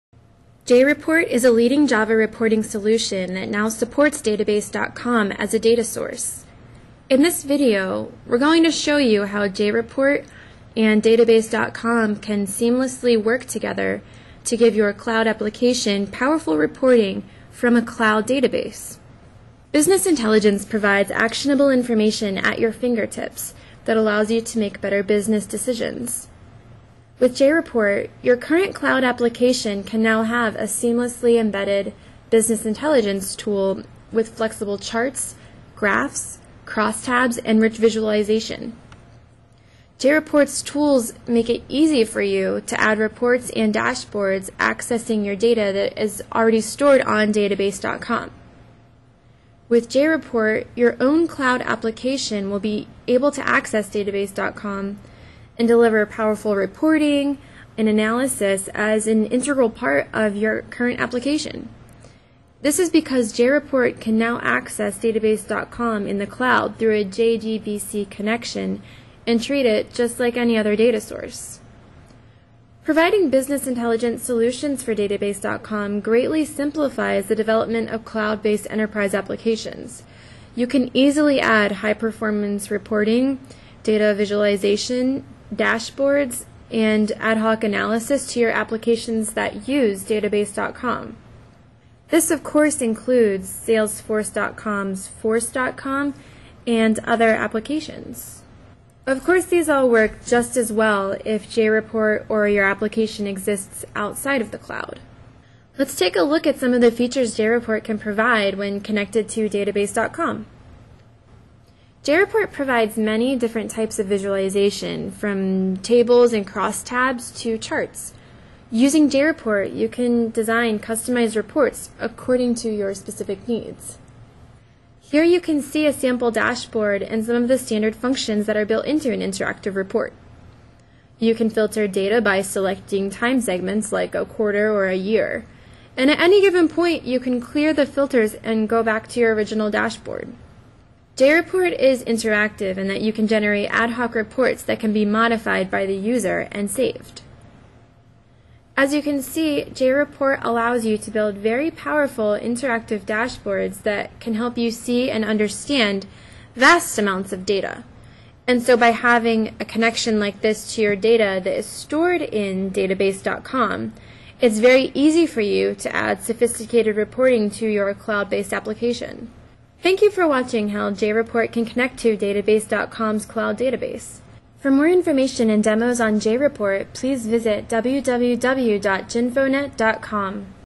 JReport Enables Cloud Reporting through Database.com
JReport, a leading java reporting tool, can now report on cloud data through Salesforce.com's Database.com. This support enables ad hoc analysis, data visualization, and high performance reporting, which help end users to interact with their data and make better informed business decisions. for more info and demos.